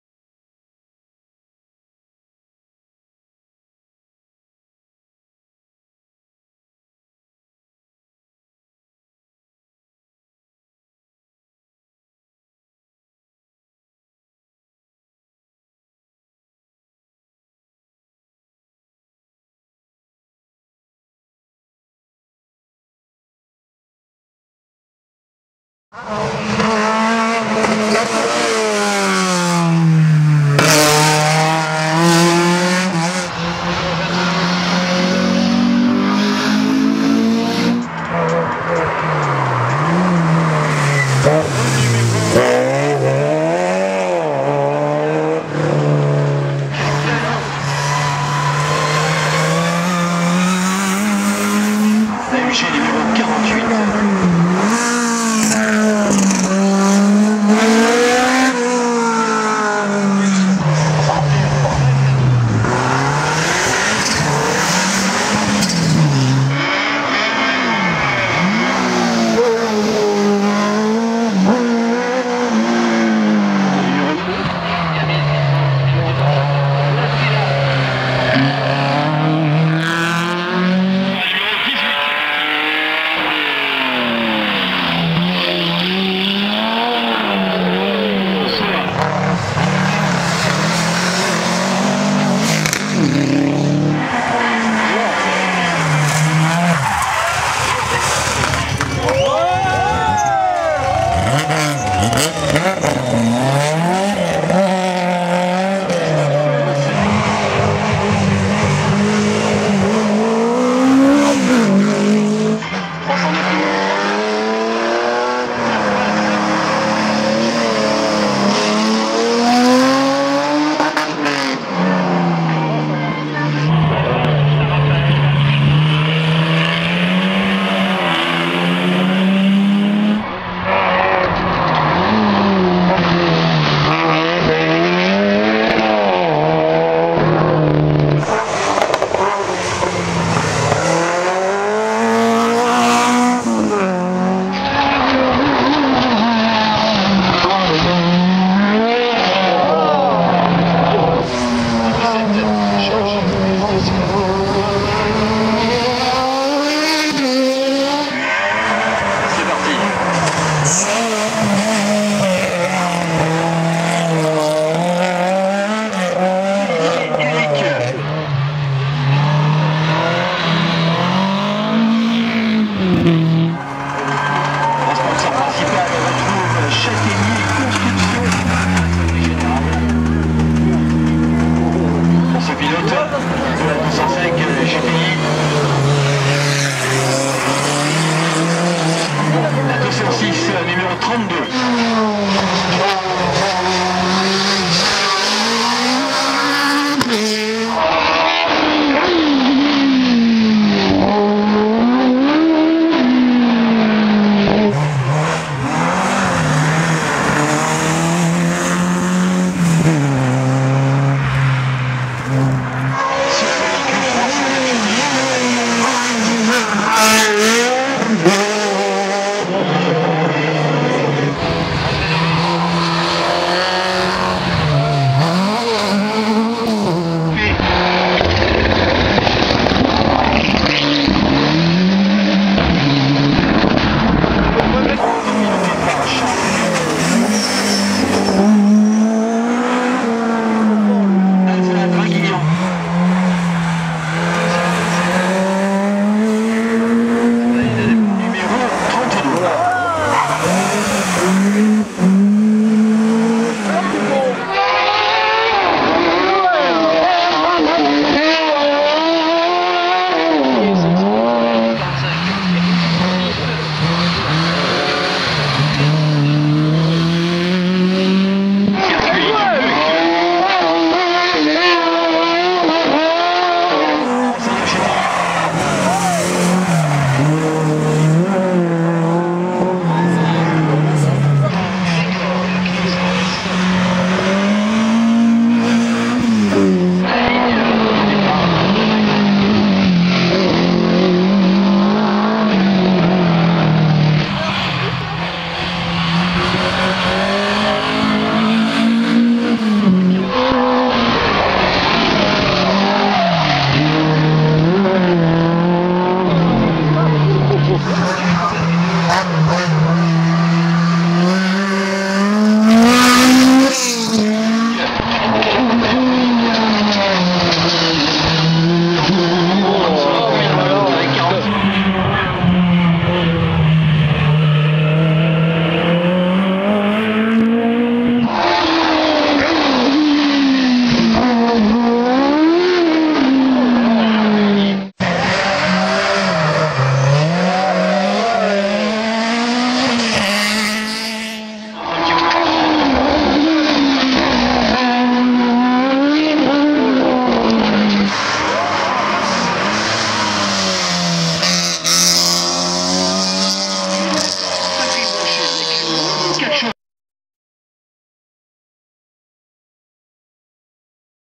Course de Côte des Planettes le Muy 2010 Show Mistakes
Vidéo de la Course de Côte des Planettes du Muy 2010
regroupant les 4 montes du dimanche (humide le matin, sec l'après midi)
filmé pour la 1ère fois avec un camescope plus appareil photo
Retrouvez nous sur Facebook pour plus de Contenus !